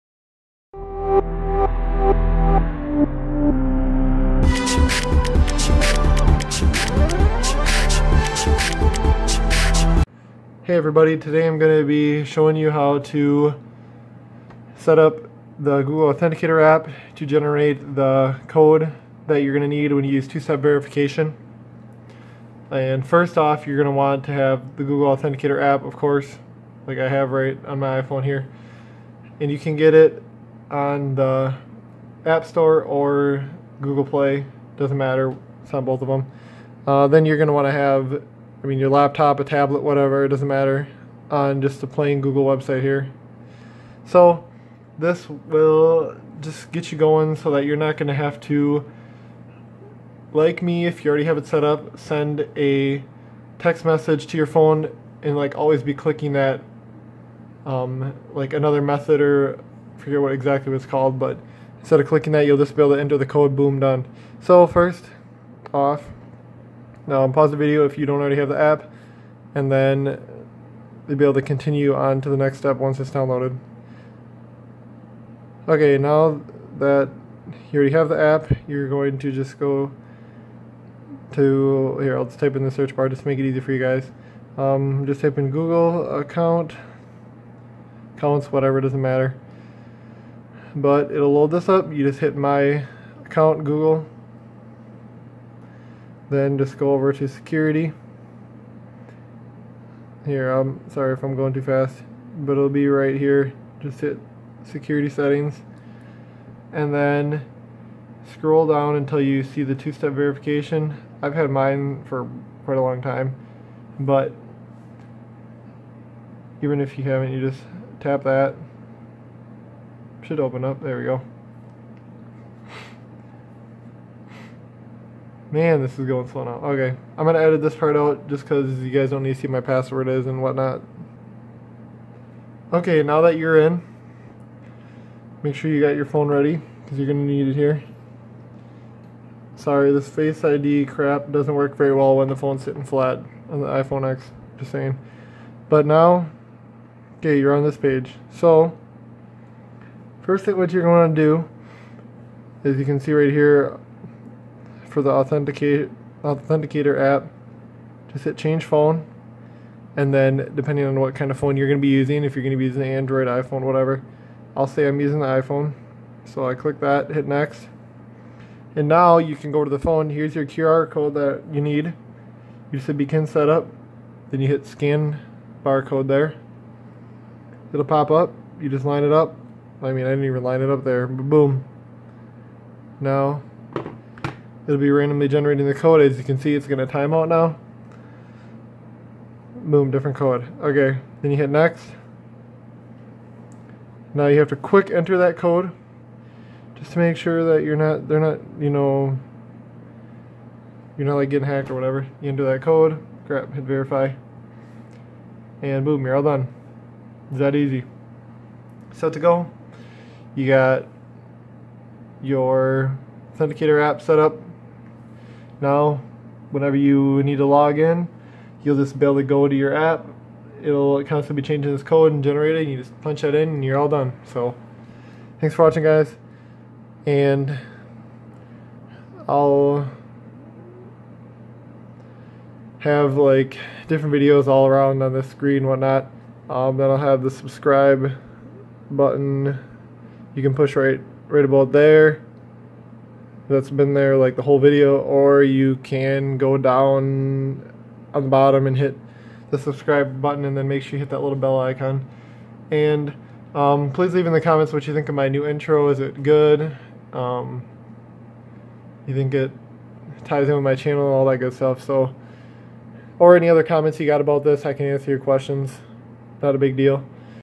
Google Authenticator Setup: Google Account.
If you got a new phone or are just setting Google Authenticator up for the first time this video will help you figure it out. I go thru how to set up an iPhone or Andriod phone. This is just for your Google account.

If you found this helpful your support is appreciated!